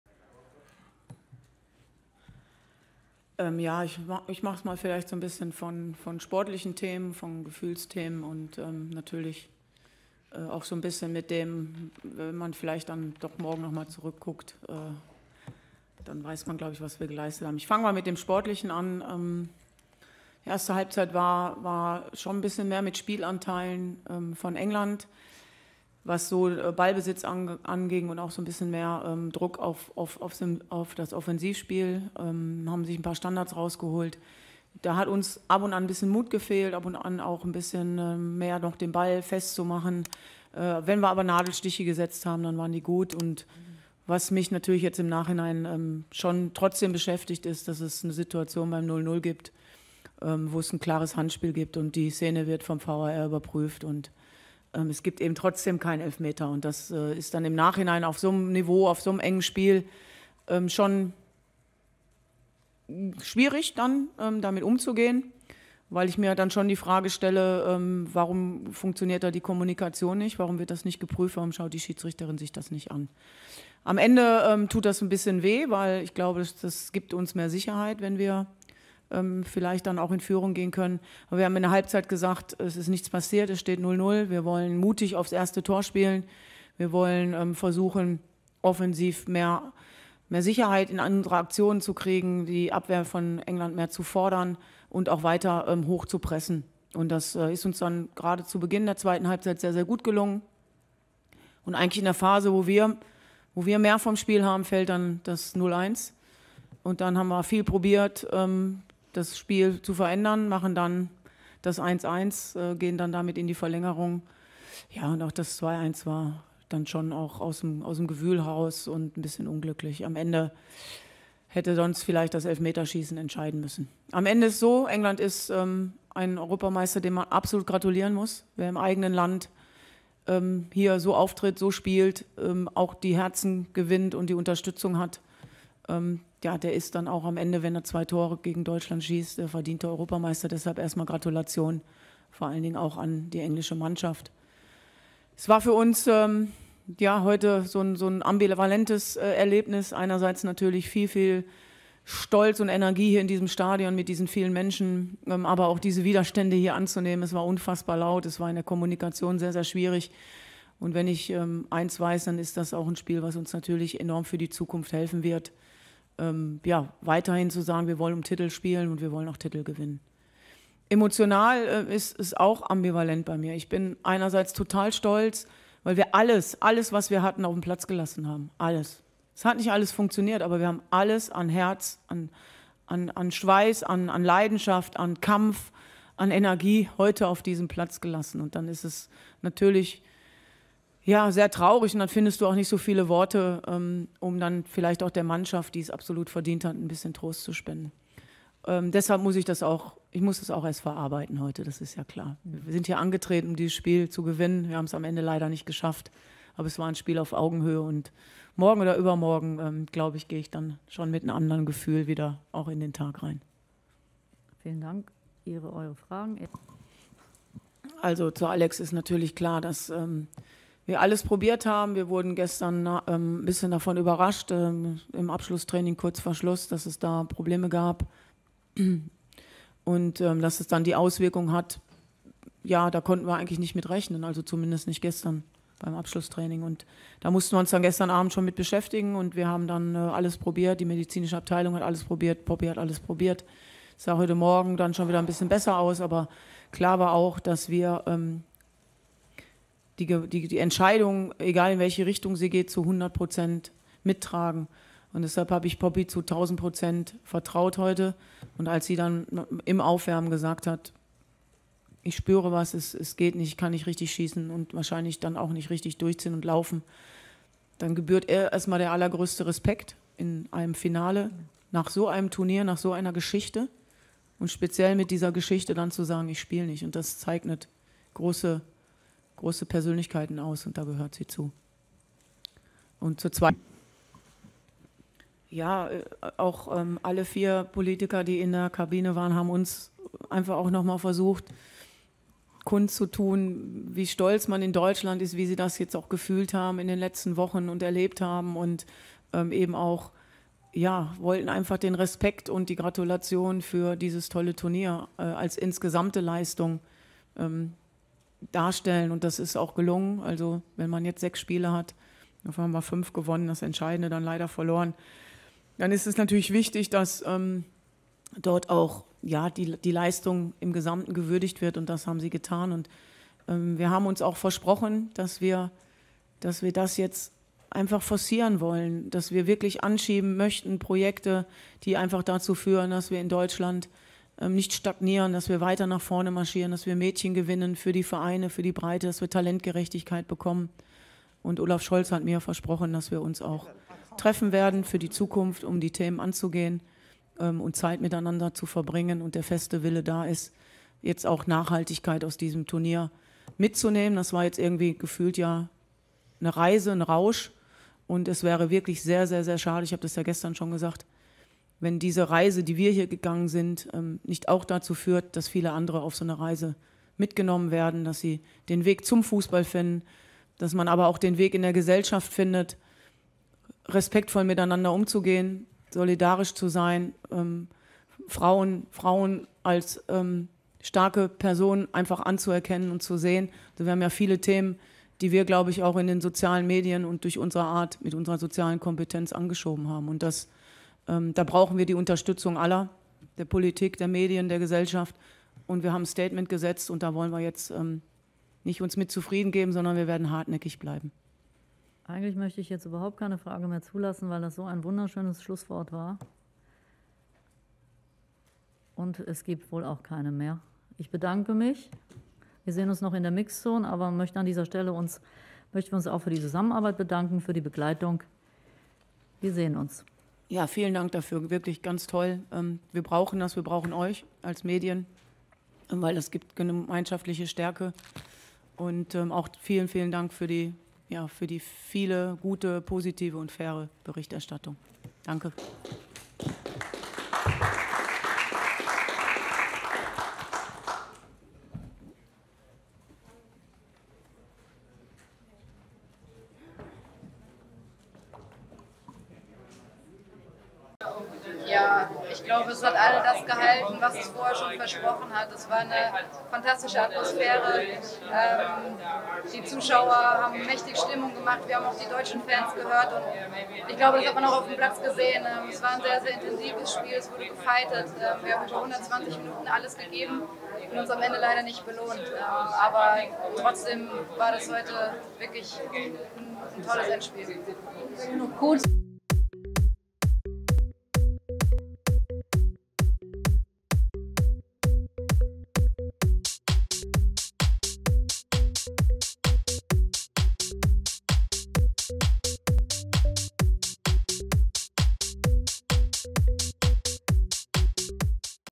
Martina Voss-Tecklenburg und Svenja Huth reagieren auf Deutschlands ersten Euro Verlust
Deutschlands Cheftrainerin Martina Voss-Tecklenburg war stolz auf die Leistungen ihrer Spielerinnen im Endspiel der Frauen-EM am Sonntag, gibt jedoch zu, dass England verdienter Meister ist.

Chloe Kelly erzielte in der 110. Minute einen dramatischen Siegtreffer für England, als die Lionesses mit einem 2:1-Sieg in Wembley ihren ersten großen internationalen Titel holten.

Voss-Tecklenburg sprach auch über die Atmosphäre eines ausverkauften Wembley-Stadions, das den Zuschauerrekord bei einem EM-Finale brach, auch bei Herrenturnieren.

00:00 Martina Voss-Tecklenburg
07:28 Svenja Huth